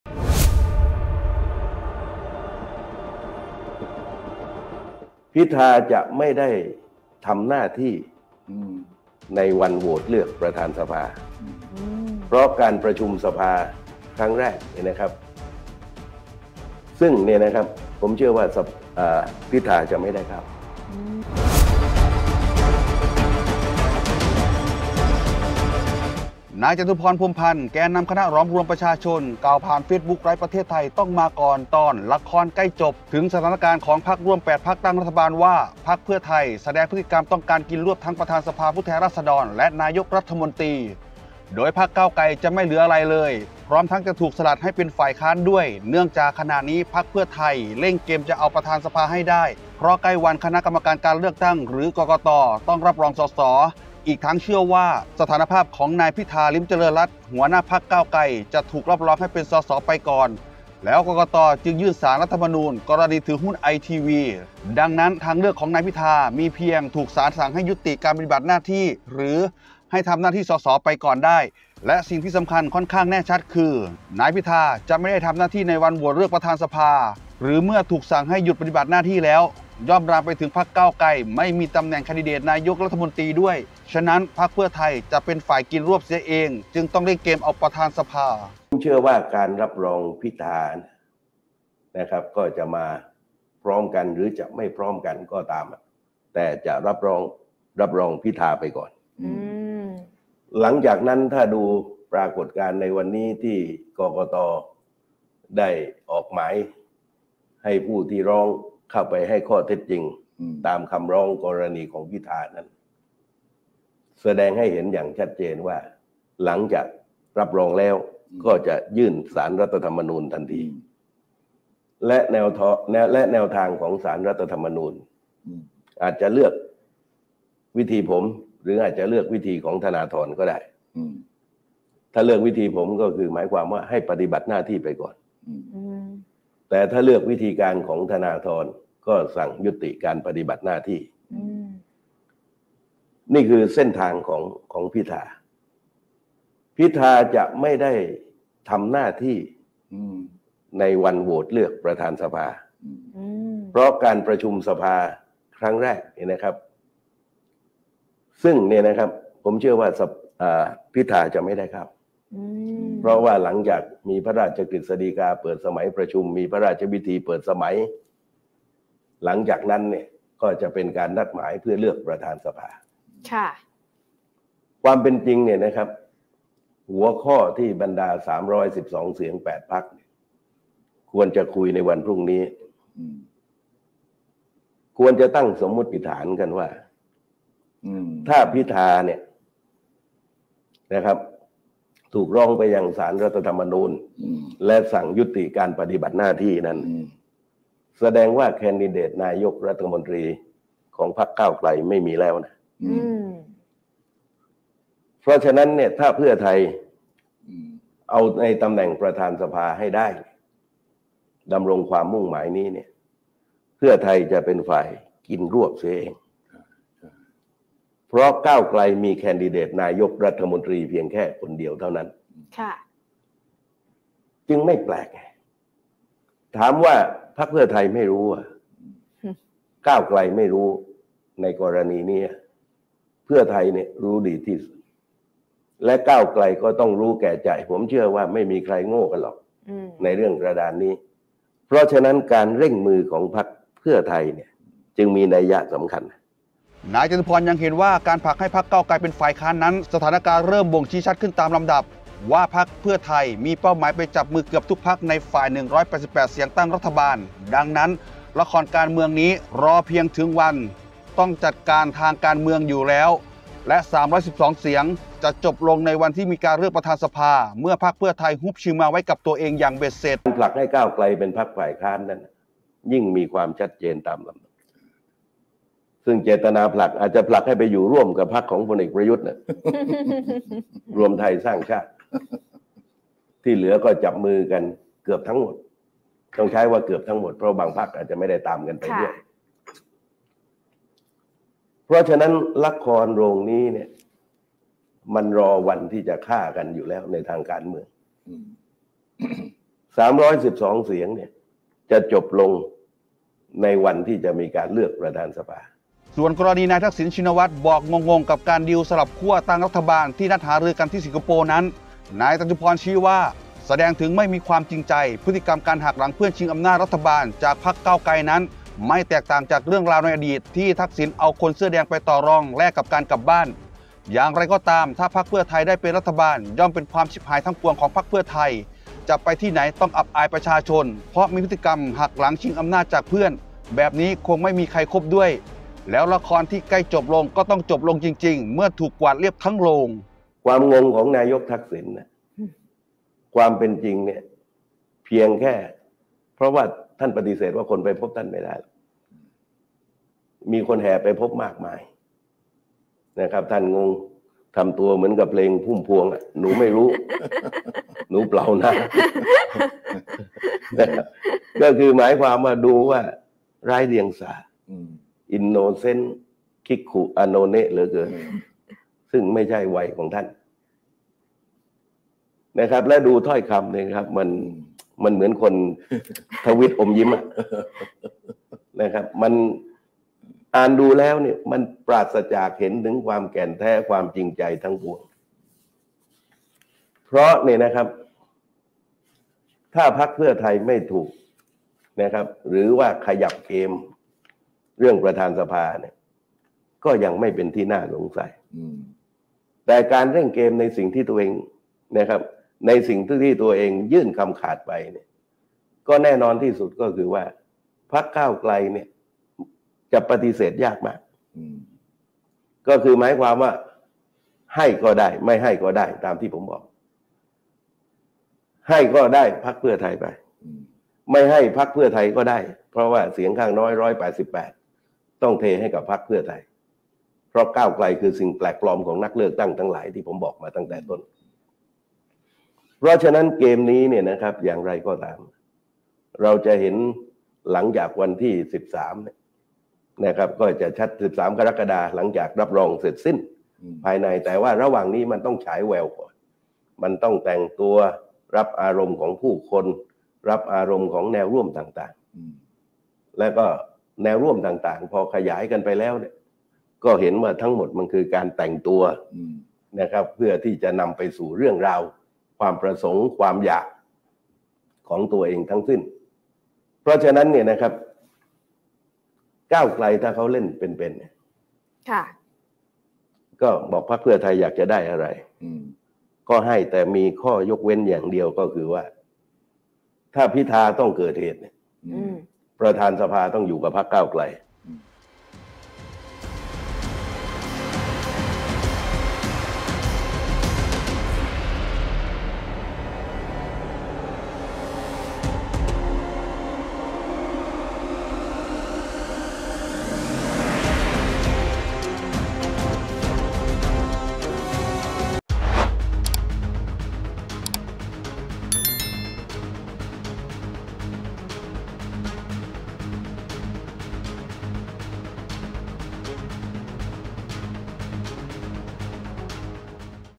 "จตุพร" อ่านไต๋ "เพื่อไทย" กินรวบนายกฯ-ปธ.สภาฯ สลัดก้าวไกลฝ่ายค้าน | HotSocial
"จตุพร" อ่านไต๋เพื่อไทย กดดันหักหลังแย่งชิง “ปธ.สภา” ตลบตะแลงรุกกินรวบถึง “นายกฯ” สลัดก้าวไกลเป็นฝ่ายค้าน เชื่อแอบจับมือฝ่าย 188 เสียงเกือบทุกพรรคตั้ง รบ. คาด "พิธา" อดเข้าสภาฯ วันโหวตเลือกประมุขฝ่ายนิติบัญญัติ